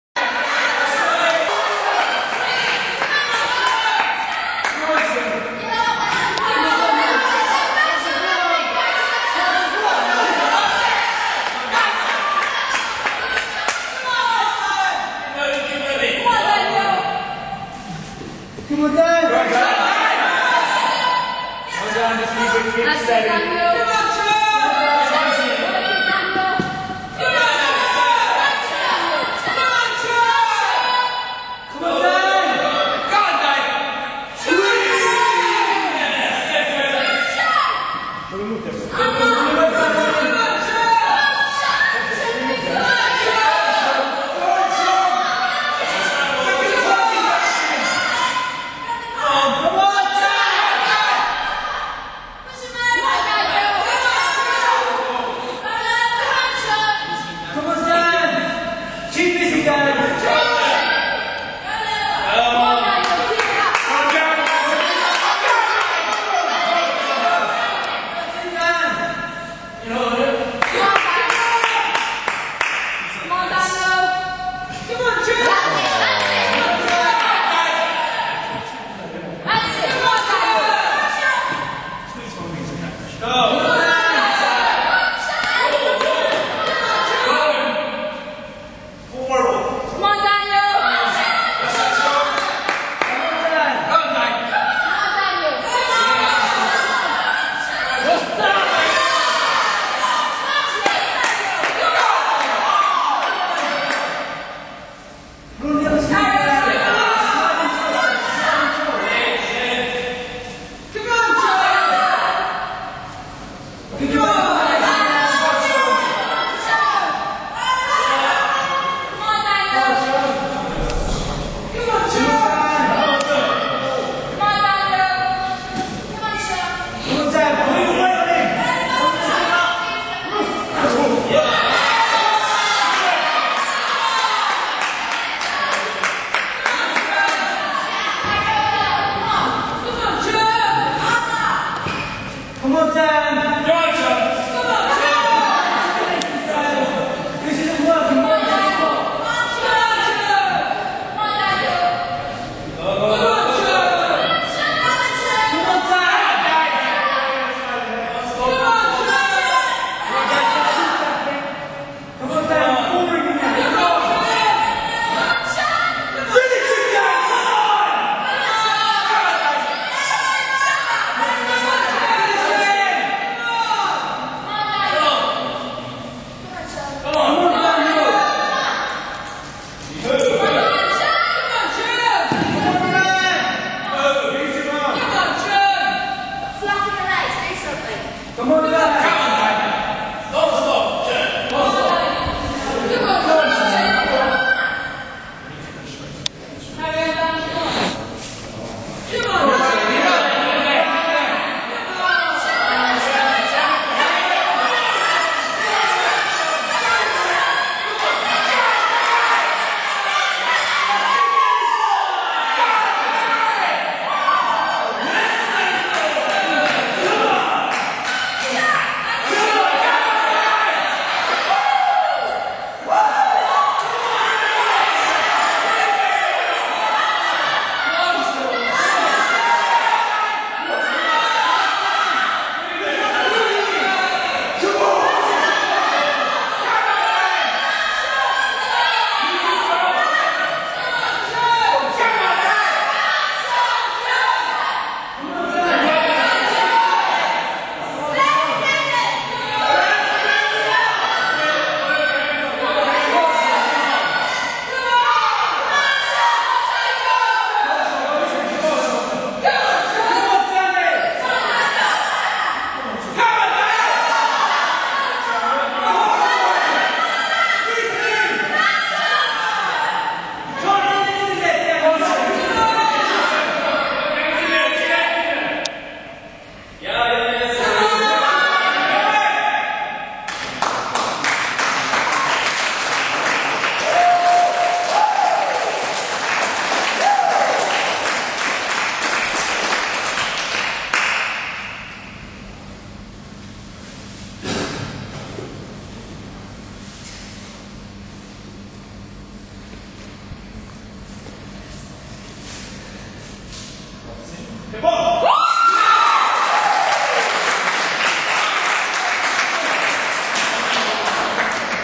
Dan vs ULU 2005
Dan "sexy boy" Thomas takes on some dude from ULU